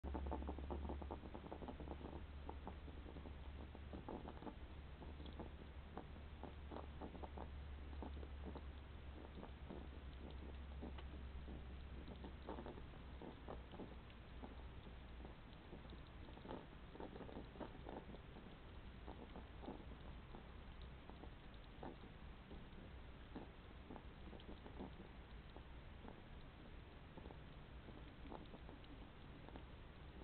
STC 0002, egel Wimpie heeft grote dorst
Deze egel-man heeft grote dorst.